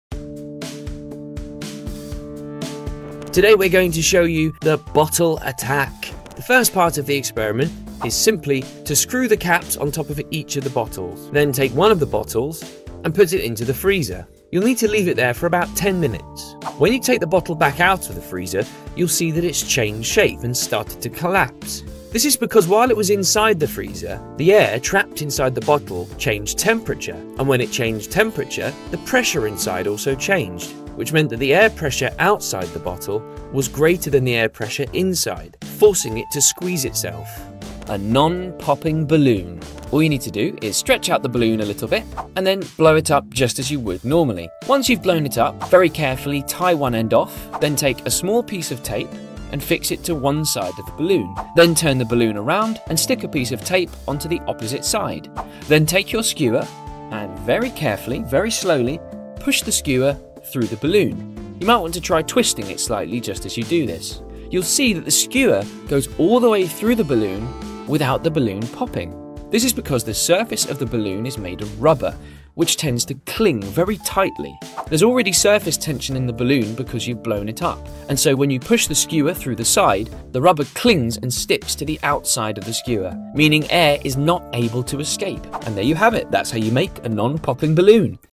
Scientists Shocked: This Balloon Refuses to Pop When Pierced
Scientists Shocked: Watch this incredible balloon defy physics as it refuses to pop when pierced! You Won't Believe your eyes as this simple experiment reveals an astonishing secret about rubber and air pressure. What if there's more to it than meets the eye?

This mind-bending demonstration shows how ordinary household items can create an allegedly impossible feat - a balloon that stays intact even when a skewer passes right through it! Using nothing more than tape, a balloon, and a skewer, viewers will discover the fascinating principles of surface tension and rubber elasticity that make this seemingly impossible trick work.

The science behind this phenomenon will challenge everything you thought you knew about balloons. From understanding air pressure to exploring the unique properties of rubber molecules, this experiment showcases the amazing world of physics in action.

Watch "Scientists Shocked: This Balloon Refuses to Pop When Pierced" till the end to unlock the secret behind this incredible demonstration!